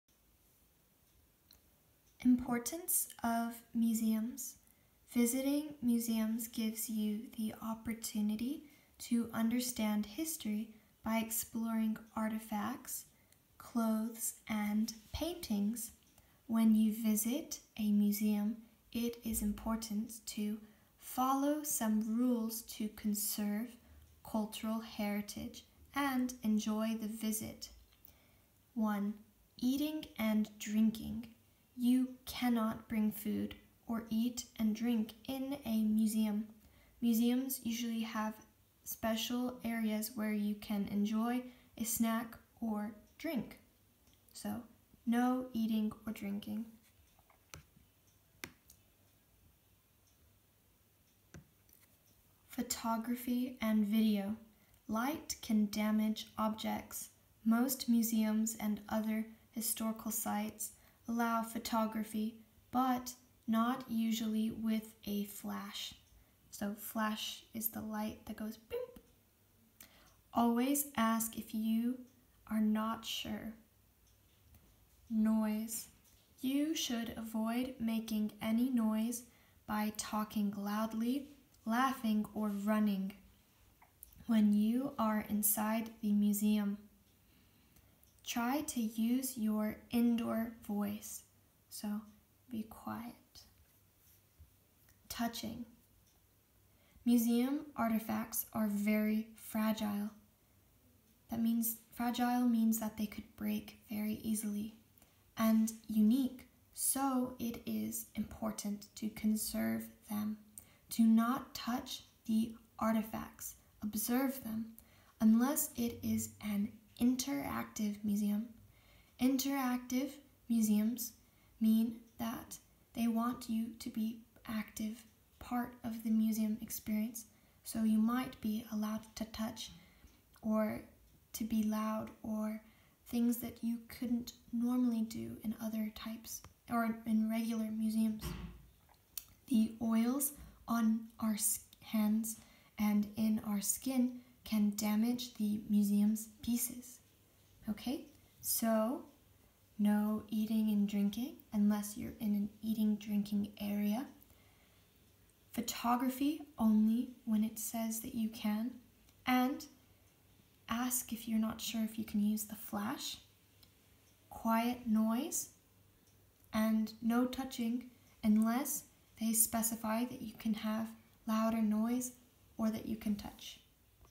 Importance of Museums
-Reading to you
-American Accent

Works Cited:
Machado de Dios, et. al. "Importance of Museums". Social Science Learn Together. Comunidad de Madrid. Primary 4